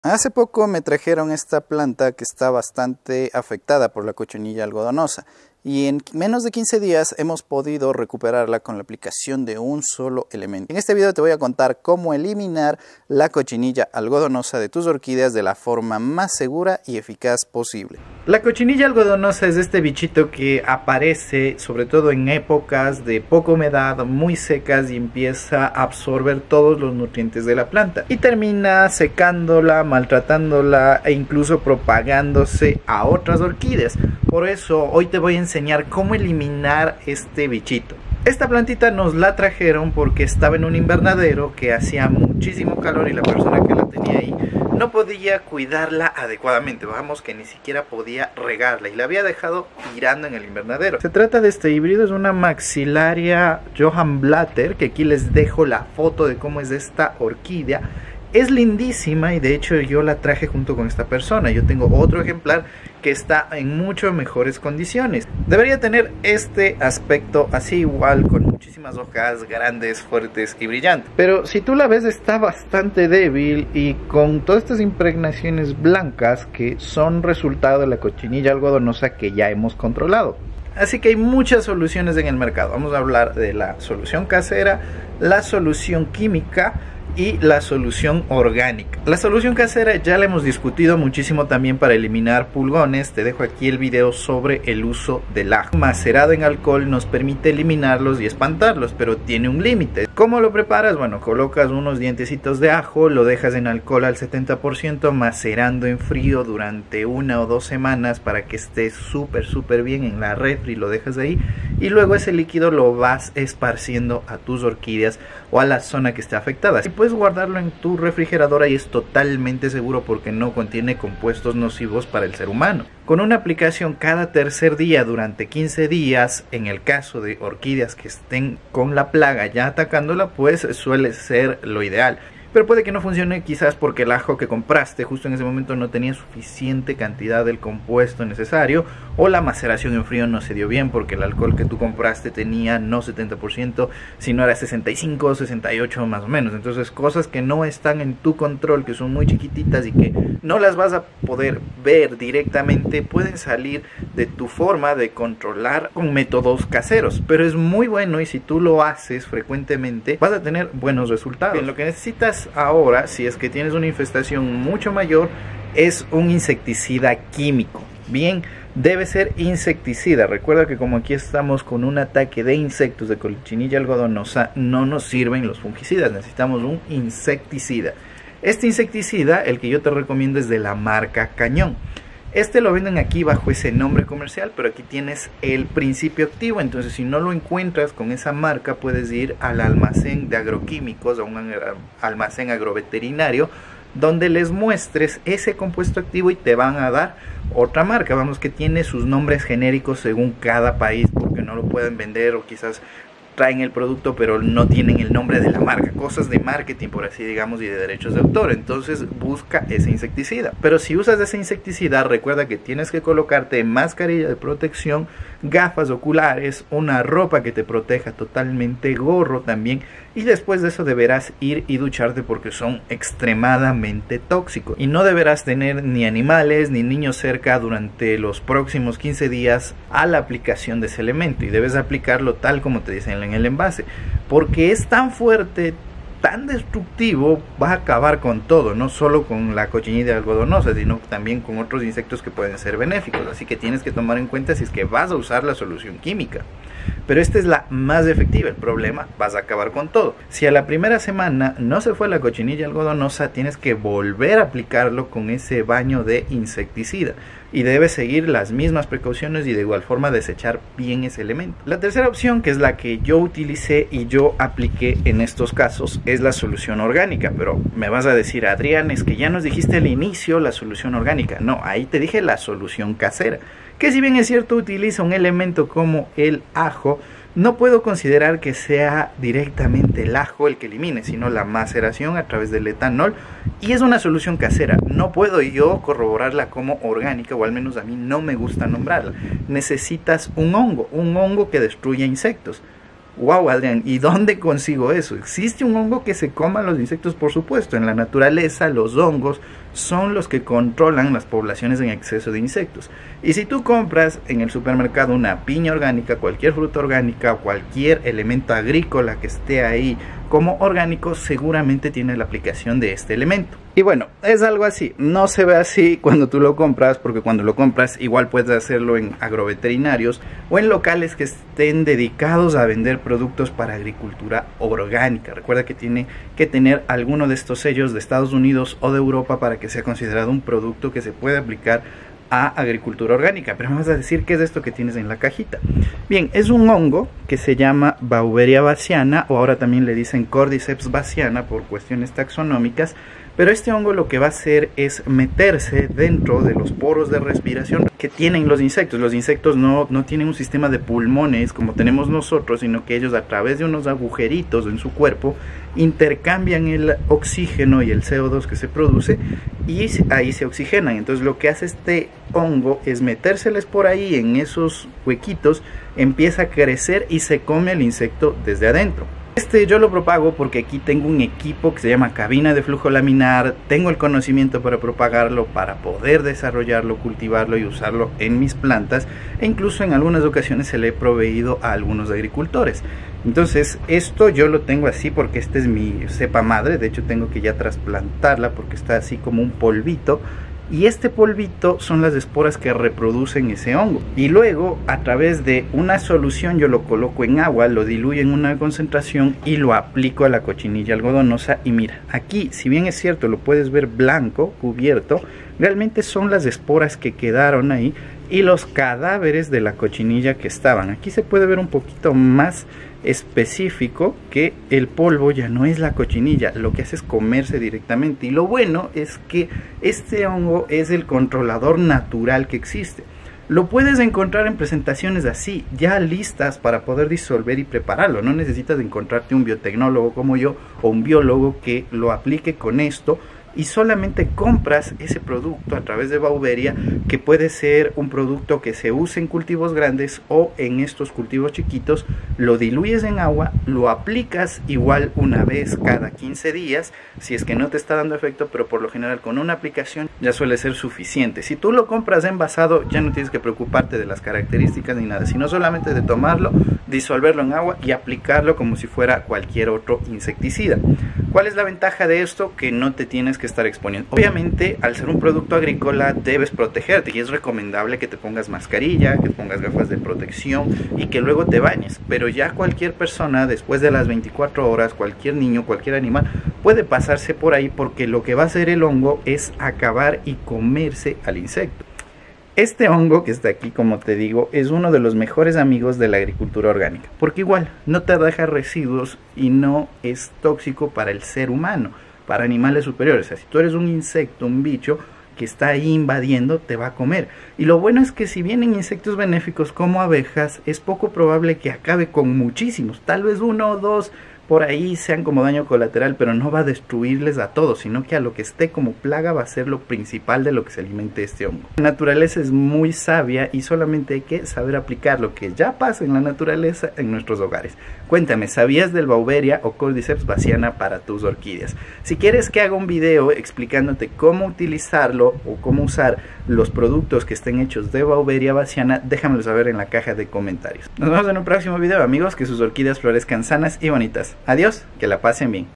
✅ELIMINA DE UNA VEZ la COCHINILLA ALGODONOSA de tus ORQUIDEAS👉🚫🐜 🌹
Hola, Hoy hablamos acerca de formas de eliminar la cochinilla algodona en sus orquídeas de forma segura y efectiva.

☛ Mis equipos de grabación:

Grabadora externa
Cámara de acción

00:00 Qué es la Cochinilla Algodonosa?
01:29 Compuesto Casero
02:44 Insecticida Químico
04:21 Insecticida Organico
08:31 La Naturaleza es sabia